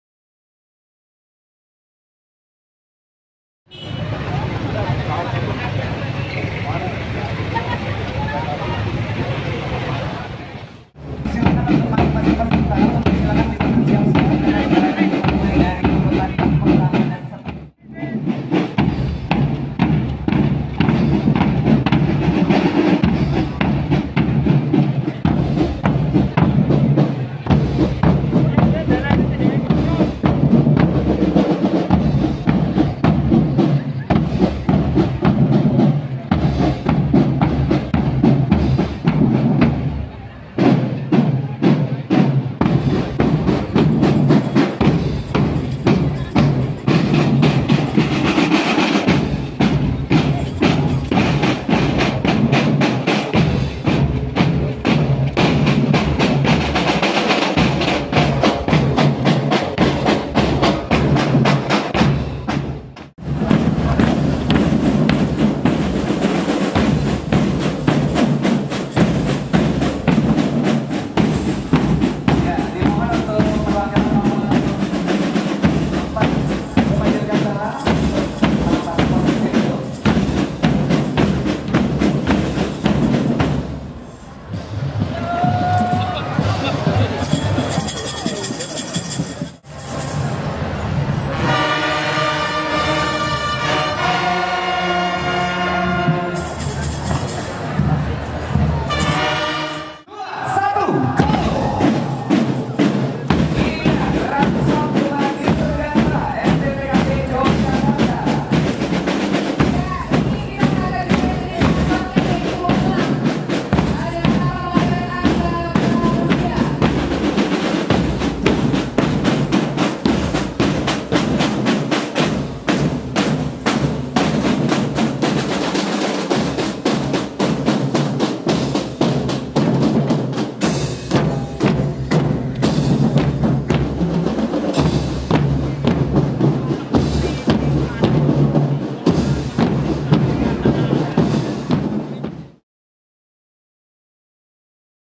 Aksi Gesit MB Gema Dirgantara pukau Pengunjung Piala Raja 2019
Aksi Marching Band Gema Dirgantara Pukau Ratusan Pengunjung Piala Raja 2019. Salah satunya adalah penampilan dari Marching Band Gema Dirgantara, Sekolah Tinggi Teknologi Kedirgantaraan (STTKD), Yogyakarta. Penampilan mereka sukses membius ratusan pengunjung di lokasi Piala Raja 2019 di jl Malioboro Jogja.

Mirip penampilan Drumband taruna AAU gesit